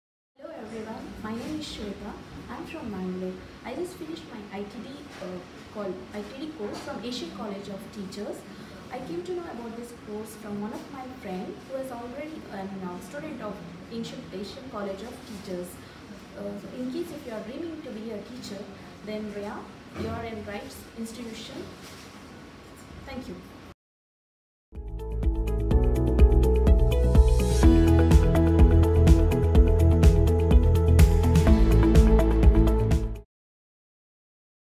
Learn about the facilities you get by doing ITD at ACT in Bangalore
Watch our Future Teacher speaks! Shweta talks about her experience of studying in International Teaching Diploma Program (ITD) 3Week Face to Face live classroom Program in Bangalore. Feeling Tired - Apply